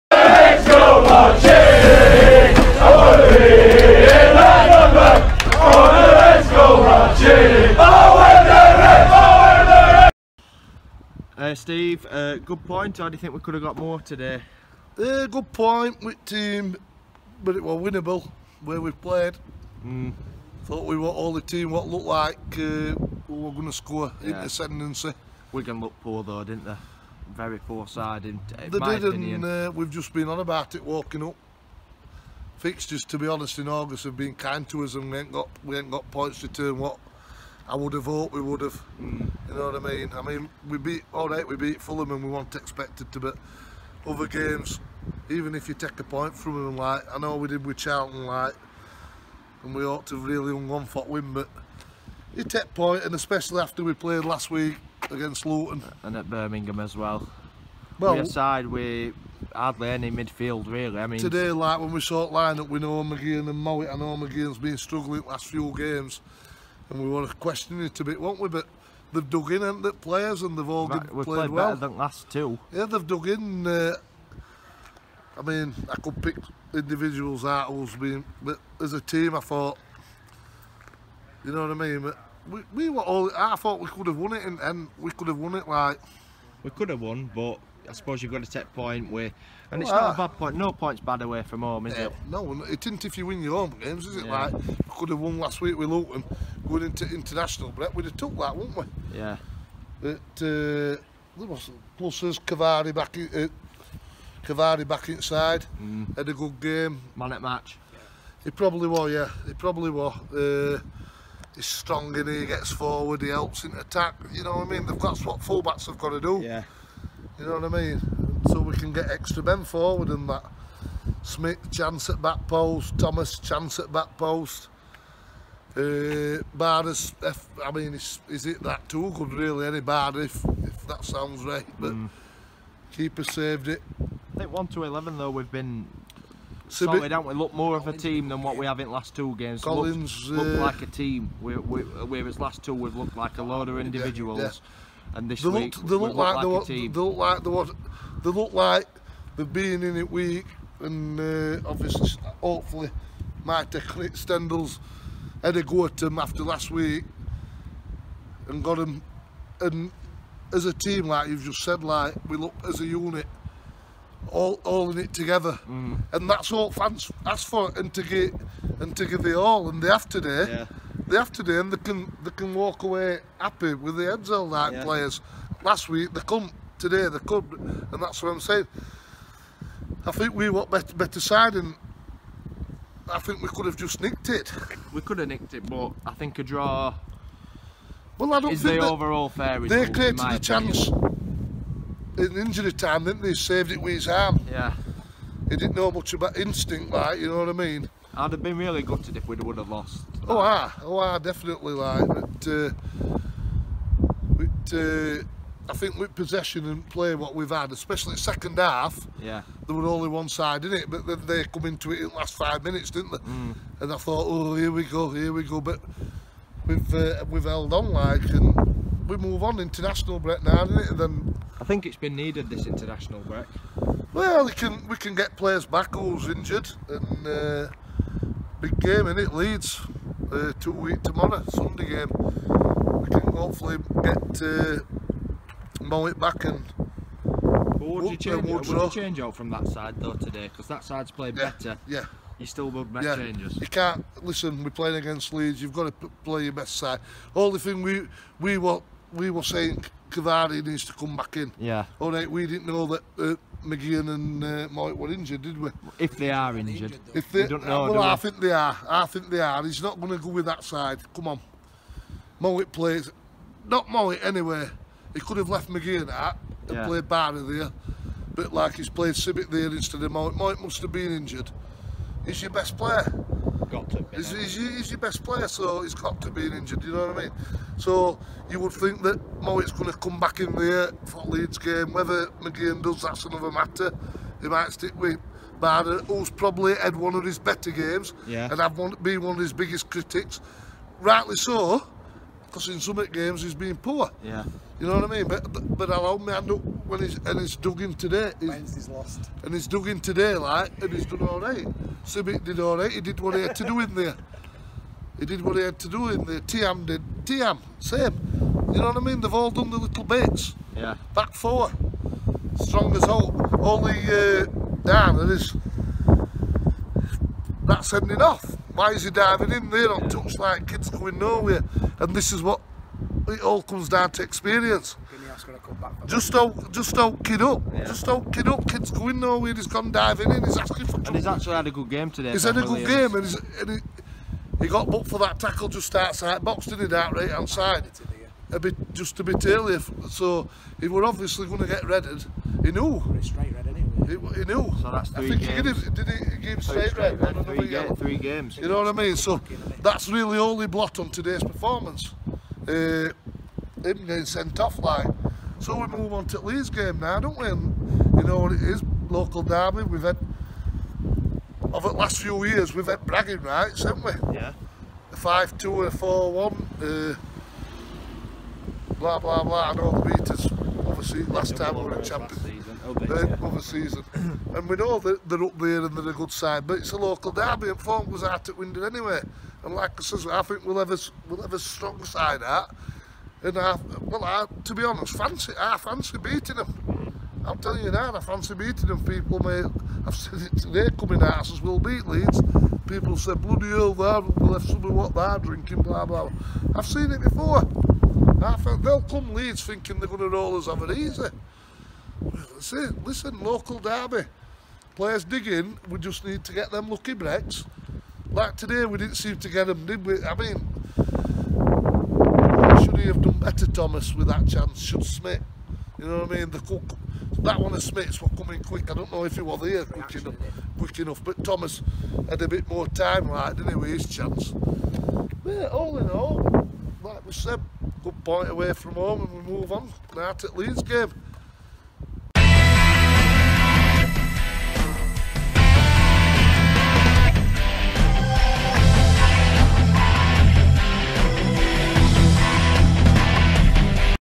Wigan Athletic 0 Barnsley 0 | Good Point But It Was Winnable! | Steve
REDS!